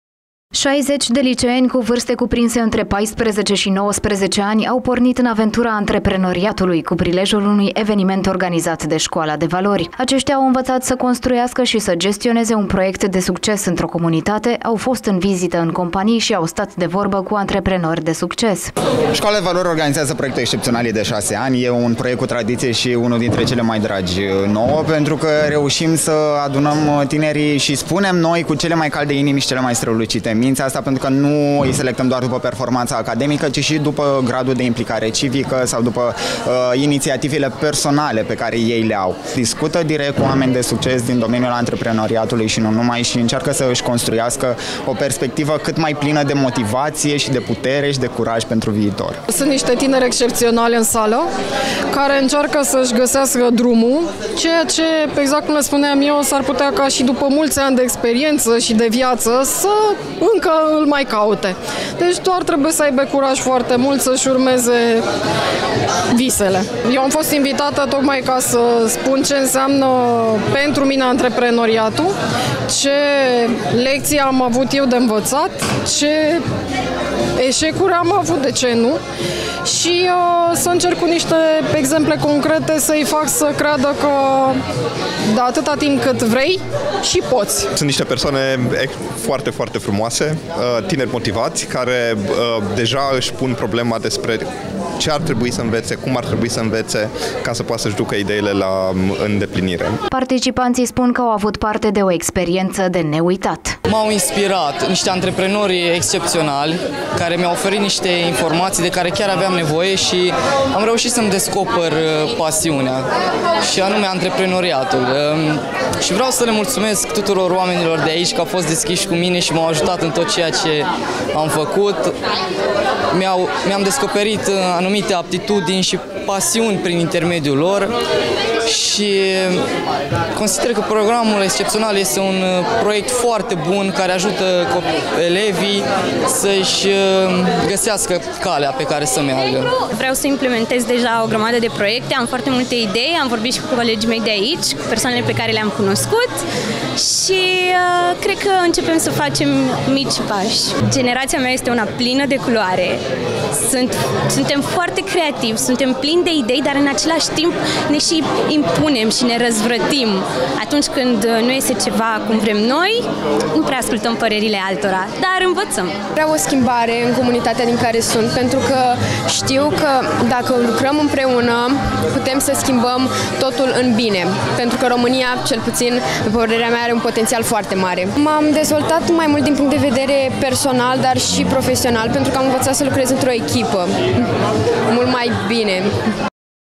60 de liceeni exceptionali au pornit in aventura antreprenoriatului
60 de liceeni, cu vârste cuprinse între 14 și 19 ani, au pornit în aventura antreprenoriatului, cu prilejul unui eveniment organizat de Școala de Valori! Aceștia au învățat să construiască și să gestioneze un proiect de succes într-o comunitate, au fost în vizită în companii și au stat de vorbă cu antreprenori de succes.Participanții spun că au avut parte de o experiența de neuitat.